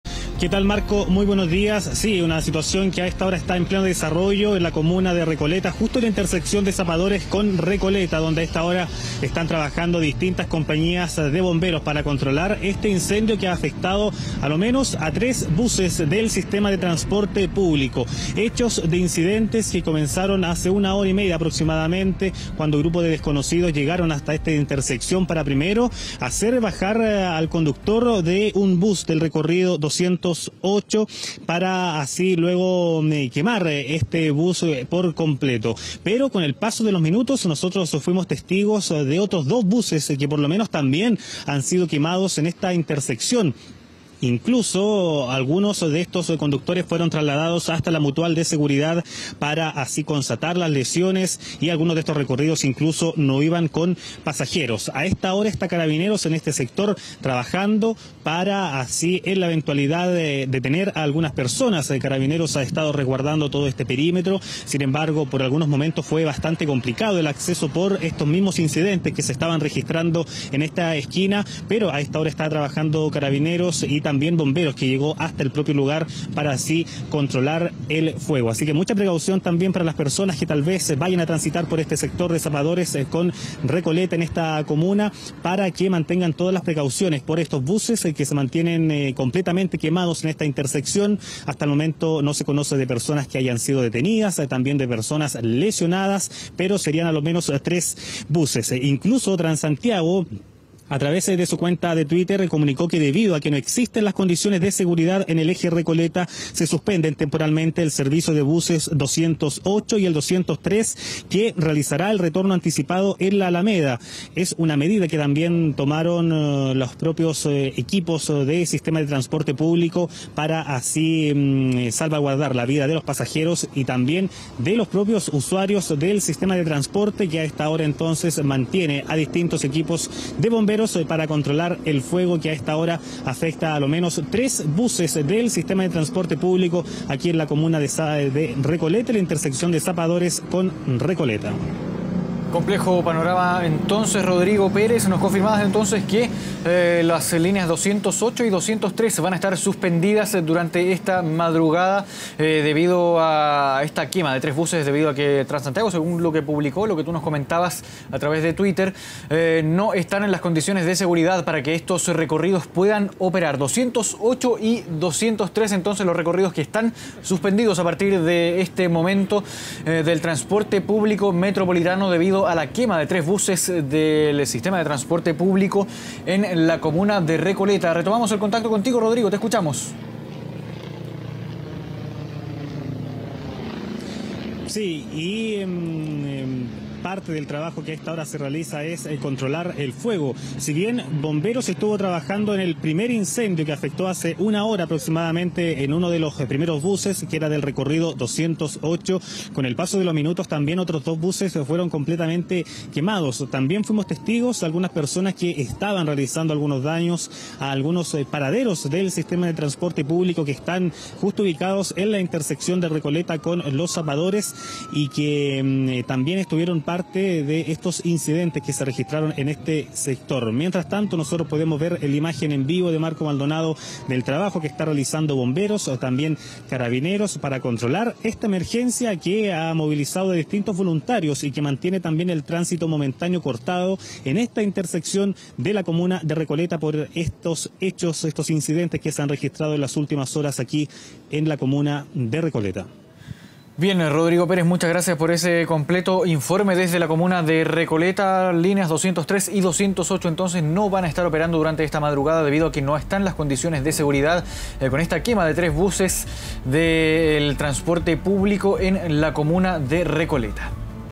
Tres buses del transporte público fueron quemados por desconocidos en Recoleta
A raíz de la situación, Transantiago decidió suspender temporalmente el servicio de buses 208 y 203.